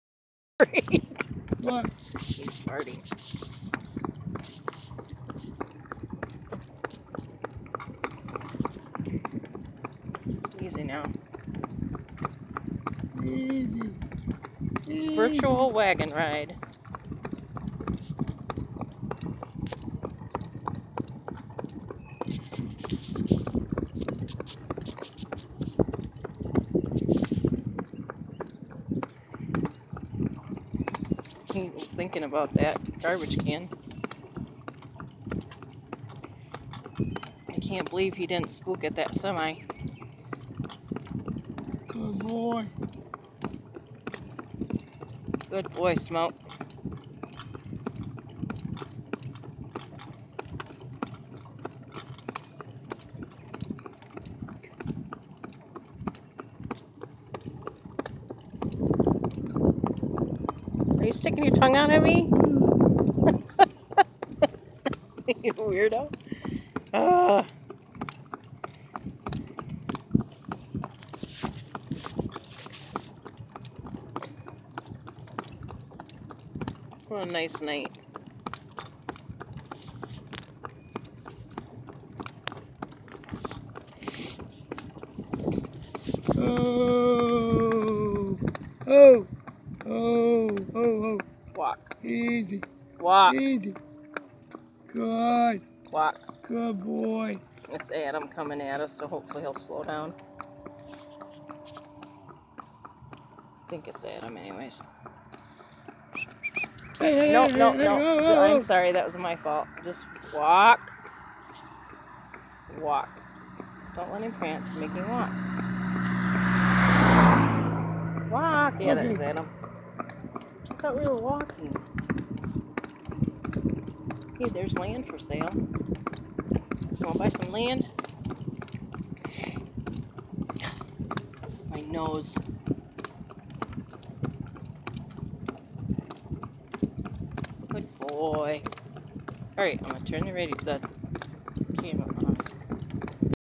North River Ranch Ponies
Driving Smokey. Practices going up and down hill's today